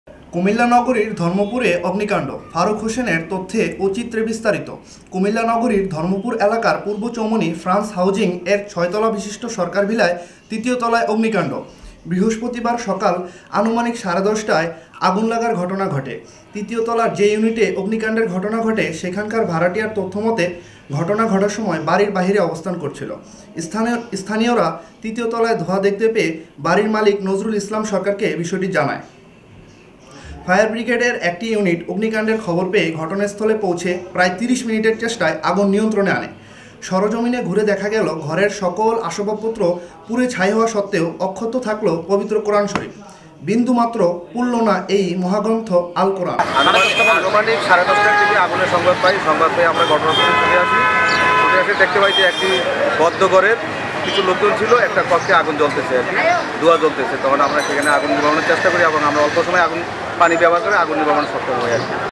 কুমিল্লা নগরীর অগ্নিকাণ্ড অক্ষত বিত্র কোরআন শরীফ
কুমিল্লা নগরীর ধর্মপুর এলাকার পূর্ব চৌমুহনি ফ্যান্স হাউজিং এর ৬ তালা বিশিষ্ট সরকার ভিলার ৩য় তলায় অগ্নিকাণ্ড। অক্ষত থাকলো পবিত্র কোরআন শরীফ

১১০ বছরেও মিললো না বয়স্ক ভাতা !

যে কোন বিষয়ে আমাদের সাথে যোগাযোগ করুন